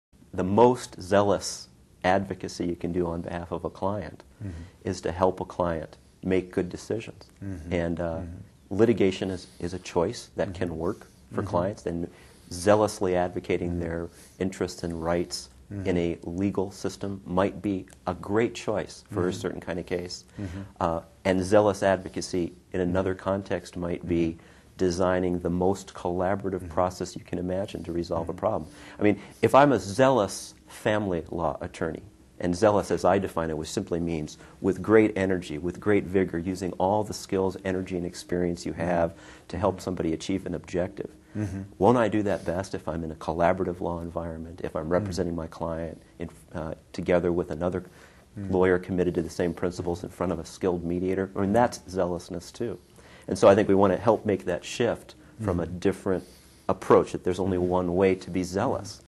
James Coben: Different Forms of Advocacy - Mediate.com Video
James Coben discusses different forms of advocacy a lawyer can have with a client. He explains that zealous advocacy might mean litigation and it might mean helping the client making the best decision through a collaborative process.